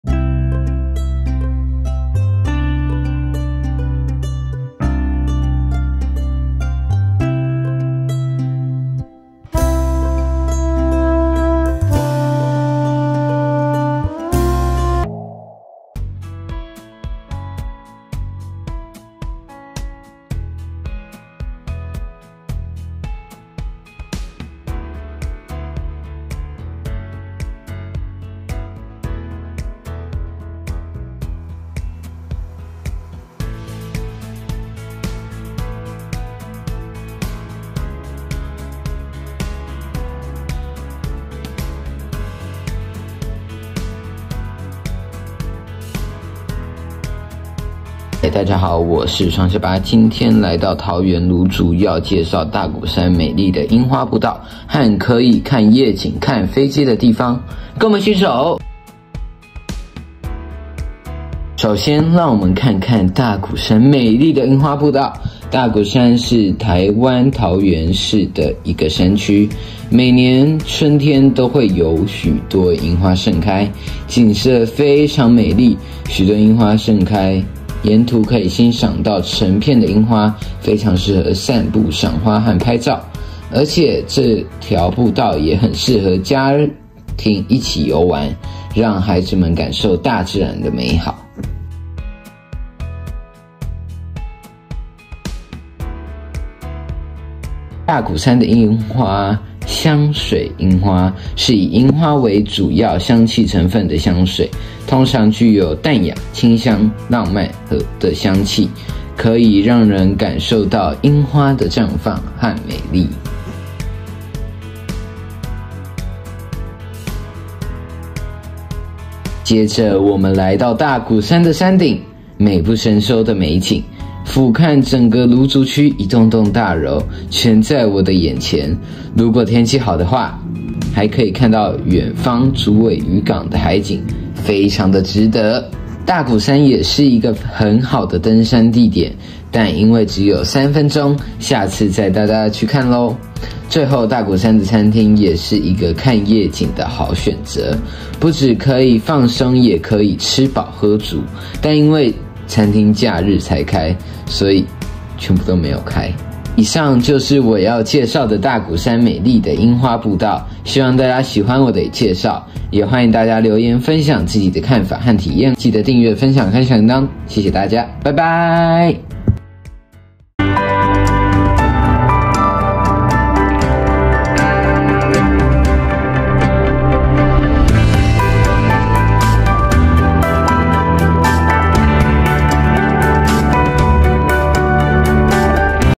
【天空之城】桃園景點來囉❗️大古山櫻花步道，從山頂俯瞰城市美景，美景美不勝收，超值得的啦❗️人生必去一次啦［3分鐘旅行x雙下巴double chin]
大古山
地址：338桃園市蘆竹區大古路228號